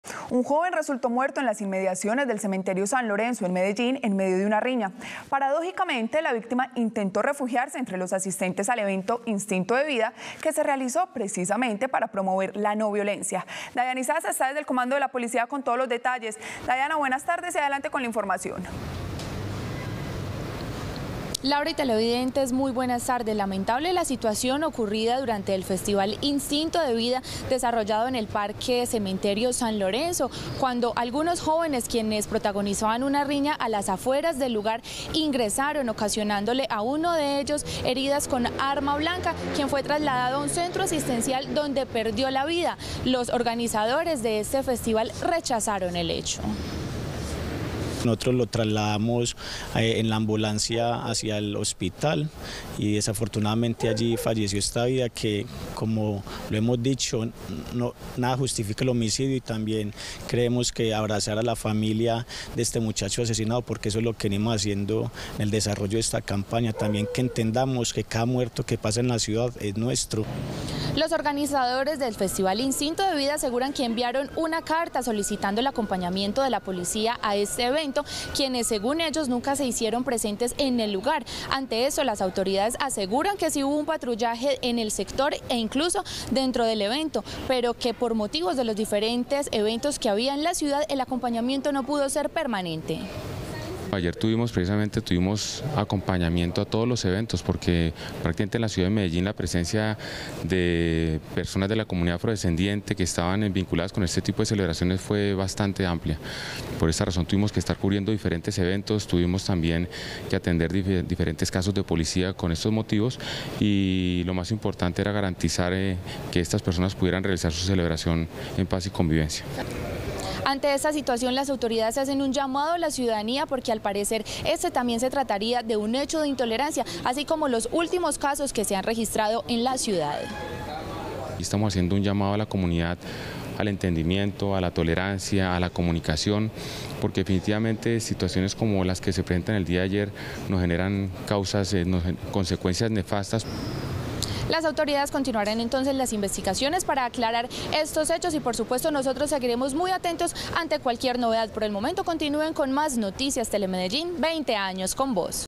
Riña en el Cementerio San Lorenzo dejó un muerto [Noticias] - Telemedellín
Un hombre de aproximadamente 20 años fue asesinado en la noche de este 14 de octubre en el barrio Las Palmas, oriente de Medellín.